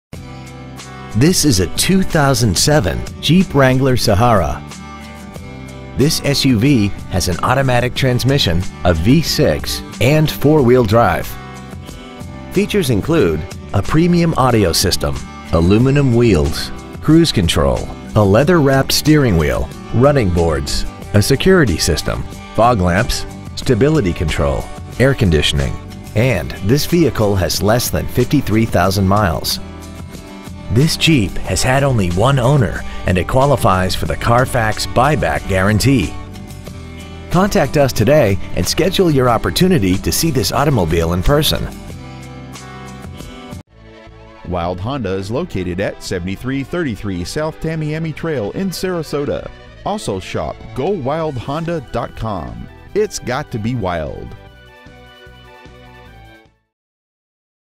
2007 Jeep Wrangler Cape Coral FL
Year : 2007
 Make : Jeep
 Model : Wrangler
 Engine : V6 Cylinder Engine
 Trans . : AUTOMATIC
 Exterior : Rescue Green Metallic with Black (Top)
 Miles : 52,528
 Interior : Dark/Medium Khaki
 Stock : 13870A

 7333 S. Tamiami Trail